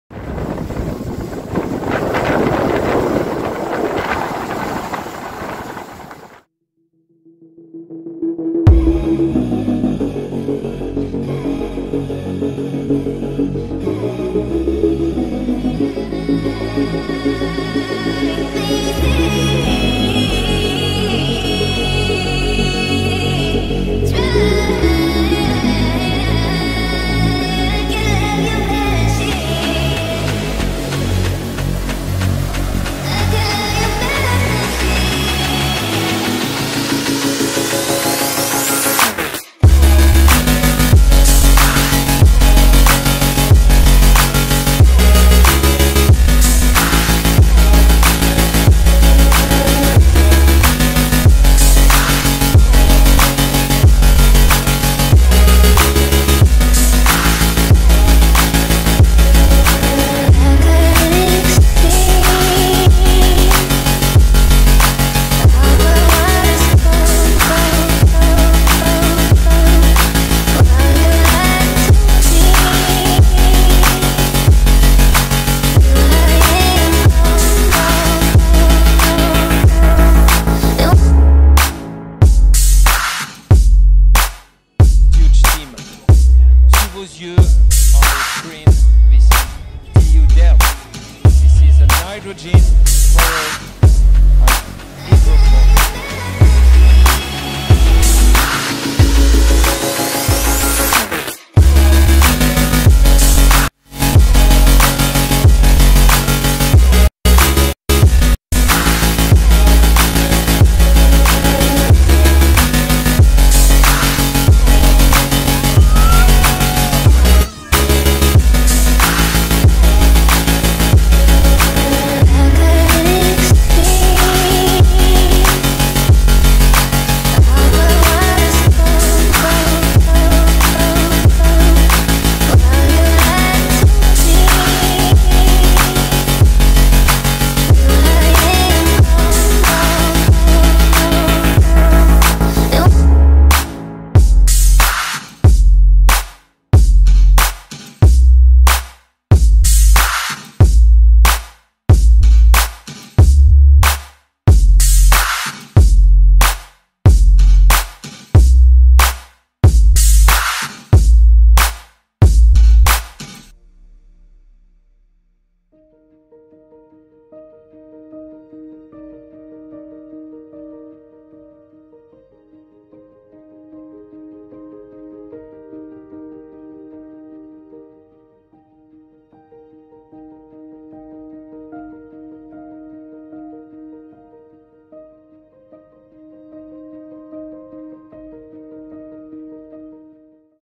The 2021 boat - worlds first hydrogen boat flying!
The team of 2021 took a giant step and made the switch to hydrogen. After using solar power for 15 years, we want to inspire the entire industry by showing what is possible with a hydrogen-powered boat.
In this video you can see this amazing boat fly!
To focus on the new technologies, the team refitted the hull of 2019 to race in the Offshore Class of the Monaco Energy Boat Challenge.

Some fun facts:
👉 The boat can reach a top speed of 40 kilometres/hour
👉 At 22km/h the boat took off and went flying above the ocean, reducing hydrogen consumption by a factor four!
👉 The total weight of the boat is 1125 kilograms
👉 The L-foils and rear strut were made of carbon fibres
👉 The boat pumps 60 litres of seawater through all the boat's systems to cool them!
👉 The tank could contain 8 kilograms of hydrogen with a pressure of 350 bar. The weight of the tank alone, 120 kg, was more than the whole boat of 2020

After this incredible year there is a new team excited to built the next hydrogen boat.
This friday at 16:30 the team working on the boat will be introduced and adding to this our big rebranding will be revealed!